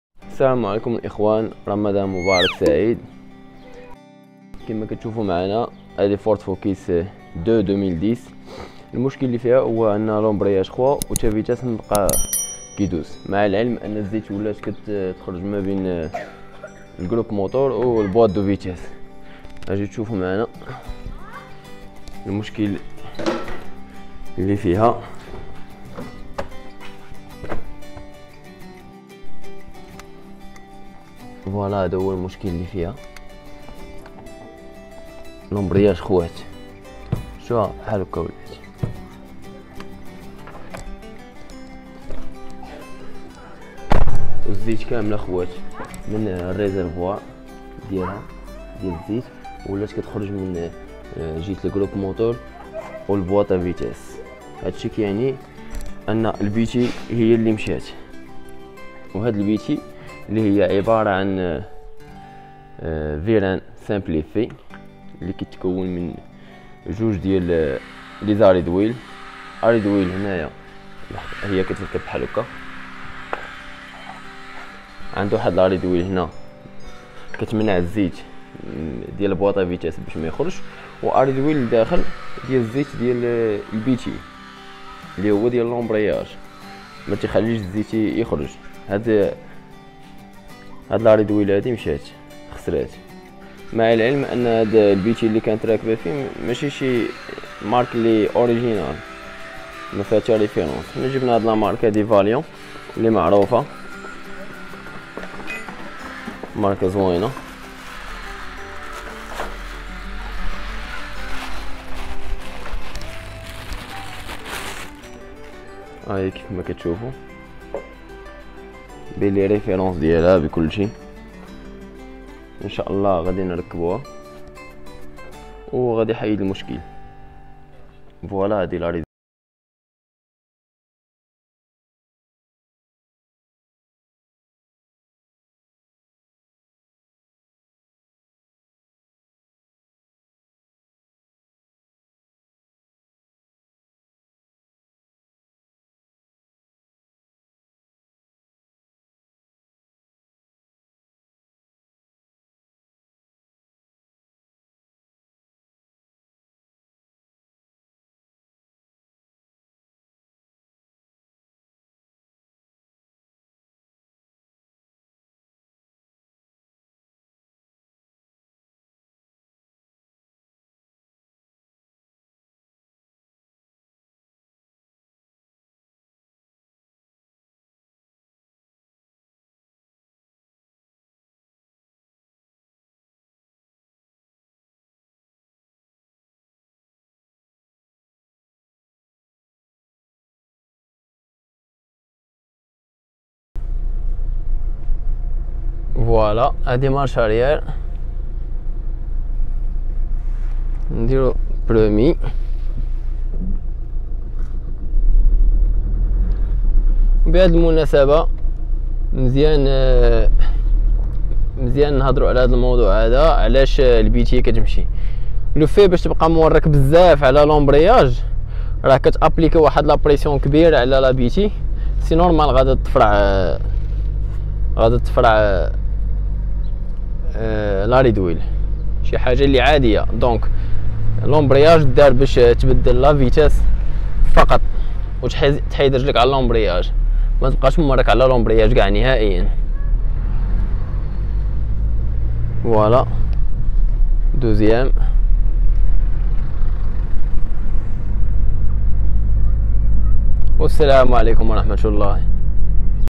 Problème D'embrayage Vide FORD FOCUS2 2010+ La solution 💡👌💪🔧🔩⚙
Le principe de fonctionnement est commun à tous les systèmes hydrauliques : plusieurs éléments sont reliés entre eux par des durites (dites durites aviation ou encore "flexible hydraulique renforcé" car très résistantes à la pression interne) dans lesquelles il y a un fluide hydraulique (en général le liquide de frein qui est partagé donc aussi par le système d'embrayage). Quand je mets sous pression ce fluide d'un côté du "tuyaux" (côté émetteur) il veut ressortir de l'autre côté (côté récepteur) ce qui émet donc une pression dans ce sens. Cette pression servira à faire bouger ce que l'on veut : des pistons d'étrier (reliés aux plaquettes) ou encore une butée d'embrayage : ce qui nous intéresse ici.
C'est un peu comme quand on appuie sur une seringue remplie, il y a un jet sous pression qui sort (là où il y a le doigt c'est l'émetteur, et côté du jet c'est le récepteur). Pression qui change selon l'intensité de la pression.